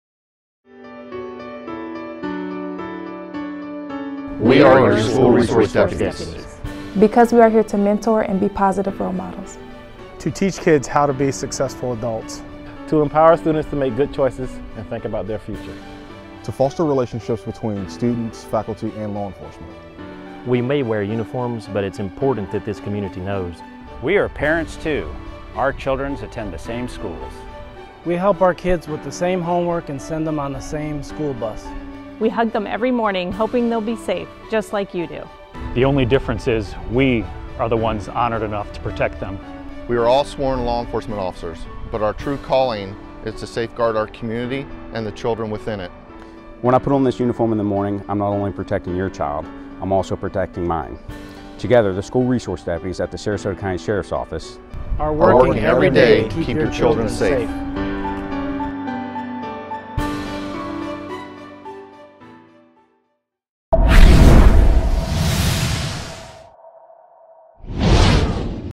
We Are Your School Resource Deputies
A message from the Sarasota County Sheriff's Office and your school resource deputies.